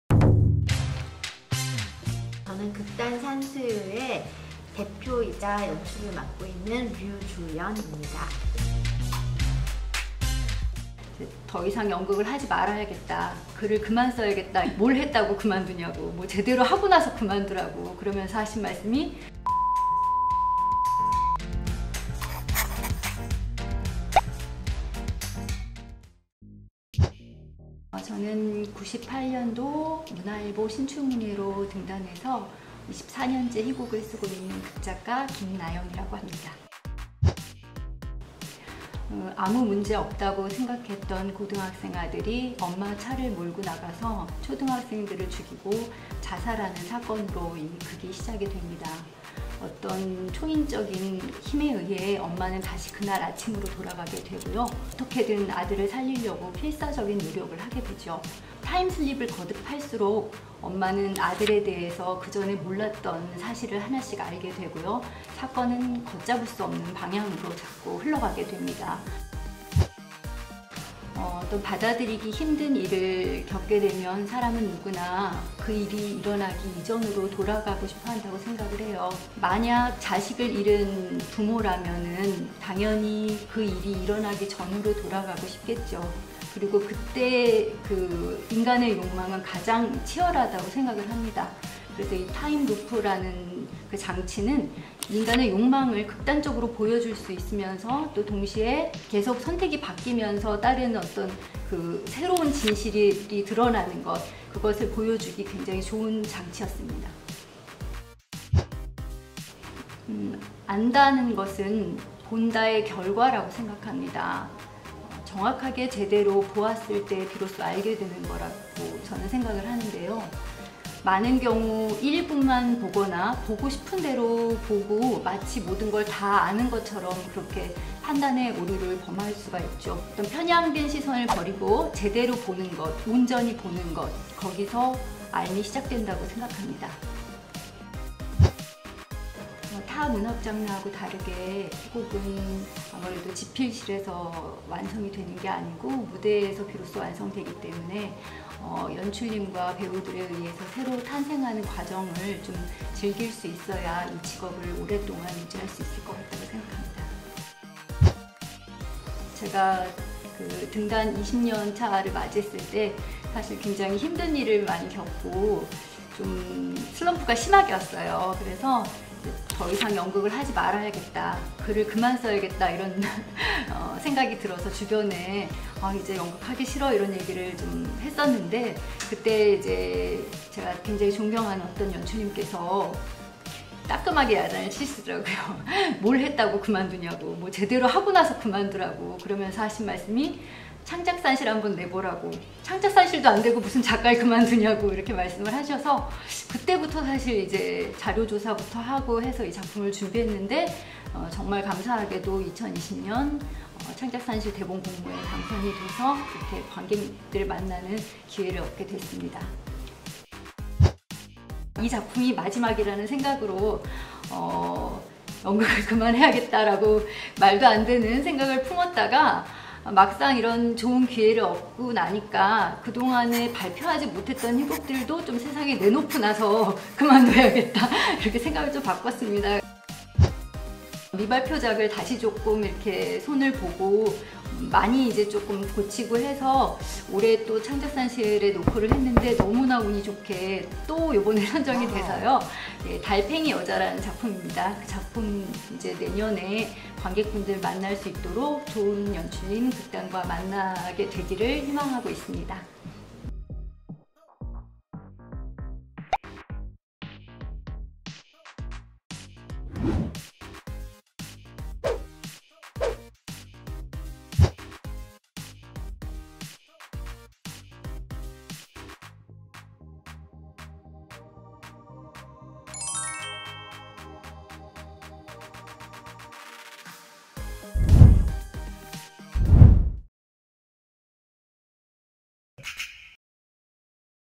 [아르코 공연라이더 현장탐방]영상 '당신은 아들을 모른다' 김나영 작가 편
'당신은 아들을 모른다'
김나영 작가 인터뷰

2020년 창작산실 대본공모 수상작!⠀⠀⠀⠀⠀⠀⠀⠀⠀⠀⠀⠀⠀

Q. 이 시대에 묻는 질문,
당신의 소통은 안녕한가요? 💭⠀⠀⠀⠀⠀⠀⠀⠀⠀⠀⠀⠀⠀⠀

2년 연속 창작산실 대본공모에 선정된 김나영 작가의 심리극,
소통단절을 파헤치는 타임루프 공연!

어느 날 미옥의 고등학생 아들 진우가
아이 둘과 아이 엄마를 차로 치어 죽이고
본인도 자살하는 사건이 일어난다.⠀⠀⠀⠀⠀⠀⠀⠀⠀⠀⠀⠀

미옥은 그저 착하다고만 생각했던 아들이
끔찍한 계획 살인의 가해자라는 사실을 믿기 어렵다.⠀⠀⠀⠀⠀⠀⠀⠀⠀⠀

진우가 어째서 그러한 선택을 했는지,
미옥은 모른다.⠀⠀⠀⠀⠀⠀⠀⠀⠀⠀⠀⠀⠀⠀⠀⠀

끔찍한 현실에 눈을 감자,
시간은 사건이 발생한 그날 아침으로 되돌아와 있다.⠀⠀⠀⠀⠀⠀⠀⠀⠀⠀⠀⠀⠀⠀⠀

미옥은 그저 악몽일 뿐이라 생각했지만,
진우가 계획을 이행하기 위해 집을 나서던
바로 그날 아침이 거짓말처럼 반복된다. 🔁
미옥은 아들을 이해하고,
그의 살인을 멈출 수 있을까? 💭
[연극] 당신은 아들을 모른다
• 공연일자 : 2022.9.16(금) ~ 10.2(일)
• 공연시간 : 평일 20:00 / 주말 15:00 (※ 월 쉼)
• 공연장소 : 대학로예술극장 소극장
• 단체명 : 극단 산수유
• 작가 : 김나영
• 연출자 : 류주연
• 출연 : 이현경, 신용진, 이지혜, 김용식, 손예리, 황비홍
• 관람등급 : 만 13세(중학생) 이상⠀⠀⠀⠀⠀⠀⠀⠀⠀⠀⠀⠀⠀⠀⠀⠀⠀⠀⠀⠀⠀⠀⠀⠀⠀⠀⠀⠀⠀

🎟 예술극장에서는 예매수수료 없이 전화예매나 홈페이지 예매가 가능합니다.